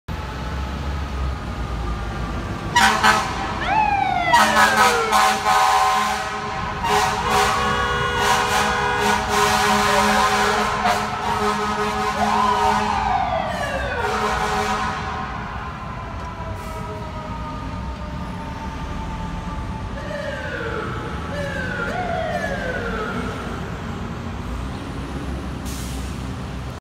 FDNY LADDER 25 & FDNY ENGINE 74 RESPONDING ON BROADWAY ON THE WEST SIDE OF MANHATTAN, NEW YORK CITY.
HERE YOU WILL SEE FDNY LADDER 25 AND FDNY ENGINE 74 RESPONDING ON BROADWAY ON THE UPPER WEST SIDE AREA OF MANHATTAN IN NEW YORK CITY.